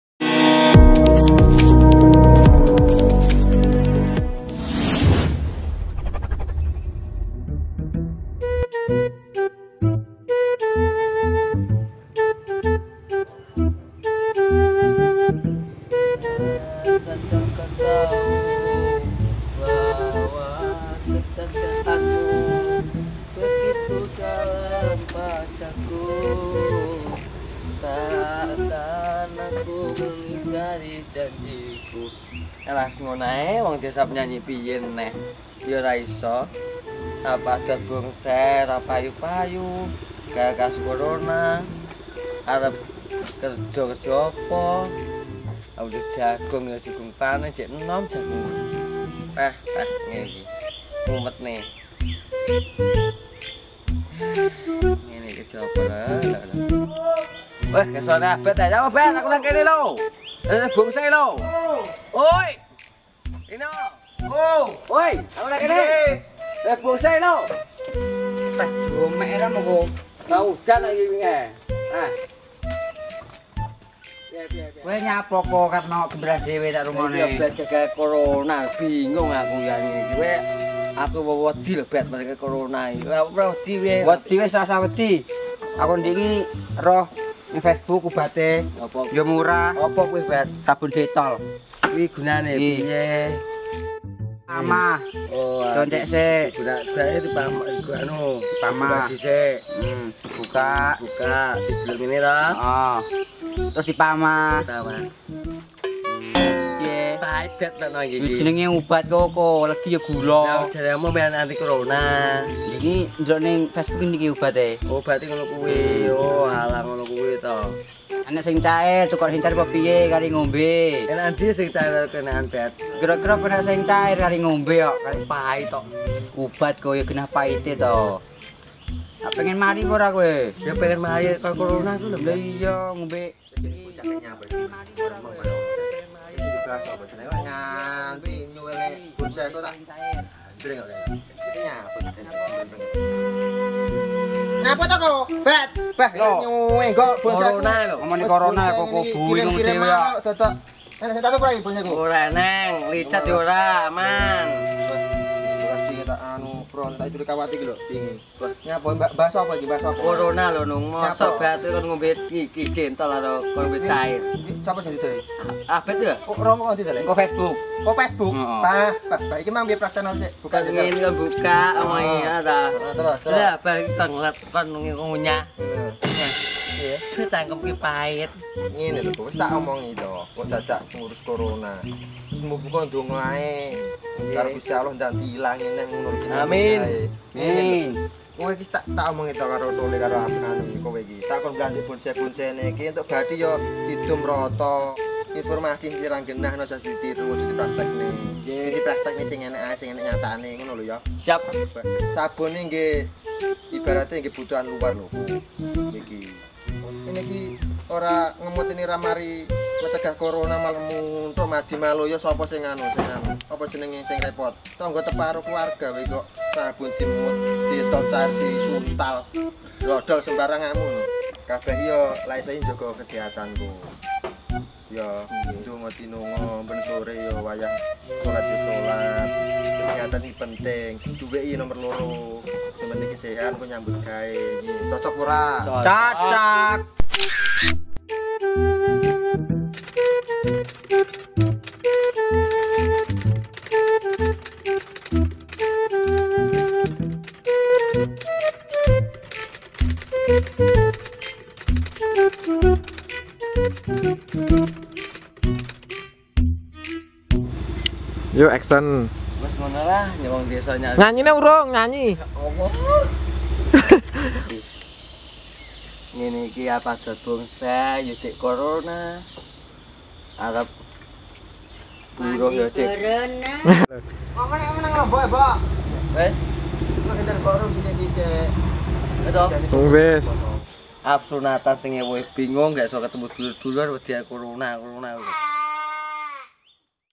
Cerita Komedi Nyata Kehidupan Desa Episode 01. "Gara-Gara Corona"
Full Playlist Semua Episode Cerita Komedi Nyata Kehidupan Desa Bisa di Tonton di

Episode 01 : "Semua Gara2 C0R0NA Koko Bingung sampai makan Sabun"

Cast:
1. Didik P (Choco)
2. Albet (Albet)
3. Reni Rio (Nung)

Kameramen + Editing
-Gaguk B.

Cerita ini dibikin berdasarkan kisah nyata yang di dramatisir tanpa naskah dan teks dialog.
Bercerita tentang Choco yang kebingungan mencari kerja ditengah Lockdown merebaknya wabah virus korona.
Padahal sebelumnya RENI RIO sudah menyuruhnya mengurus bonsai yang jika laku keuntungan akan dibagi rata.Tapi tetap saja Choco khawatir dan kebingungan dengan wabah yang menghantui.

Albet yang termakan informasi hoak dimedia sosial malah memberikan cara menangkal virus yang justru membahayakan.
Selengkapnya monggo ditonton videonya boss Q..
mohon maaf kalau tidak sempurna,karena ini baru pertama bikin dan sekedar iseng,daripada stay at home tidak ada kegiatan apa2.